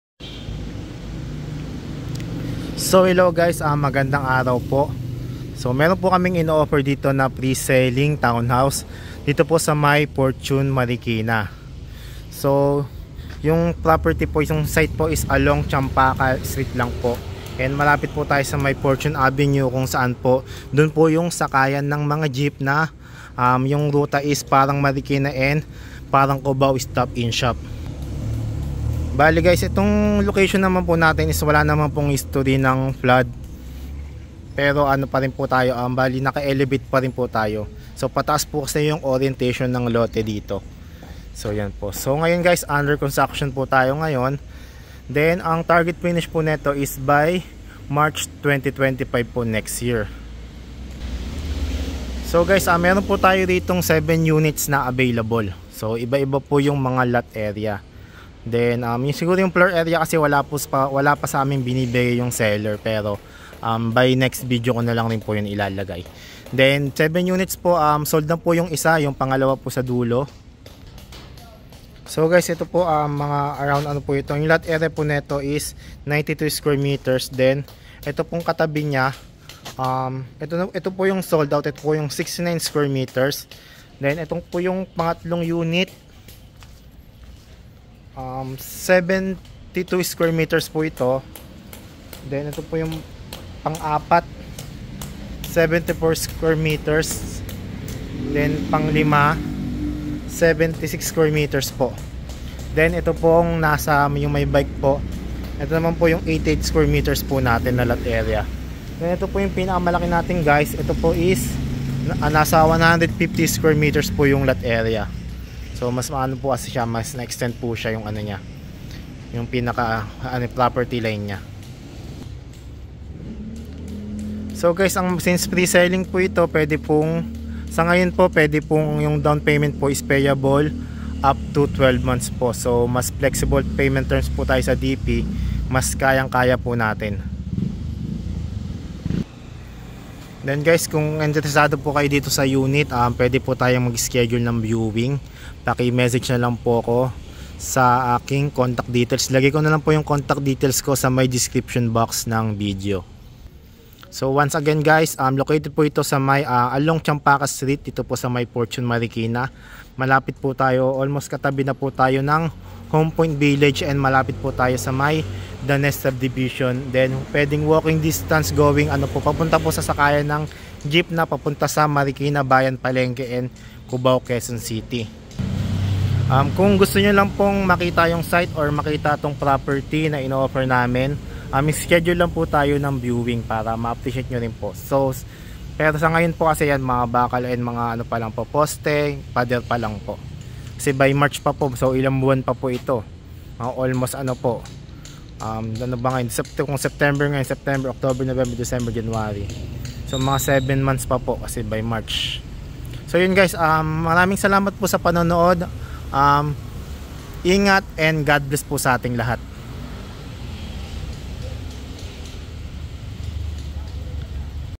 7.2M || Pre-selling House and Lot for Sale in Champaca Fortune Marikina City Flood Free
7.2M || Pre-selling House and Lot for Sale in Champaca Fortune Marikina City Flood Free

3 Bedrooms
3 Toilet and Bath
Maidsroom
1 Carport

20% Downpayment
80% Bank Financing

Target Finish: March 2025

10% DP Sample Computation
(Security Bank)

Price: 7,200,0000
10% Downpayment: 720,000
Payable in 12 mos (PDC)

90% Loanable Amount: 6,480,000
Monthly Amortization

20 Years: 53,197.47
Income Req.: 177,324.89

15 Years: 60,994.67
Income Req.: 203,315.56

Daniel M. Orias
EDJ One Realty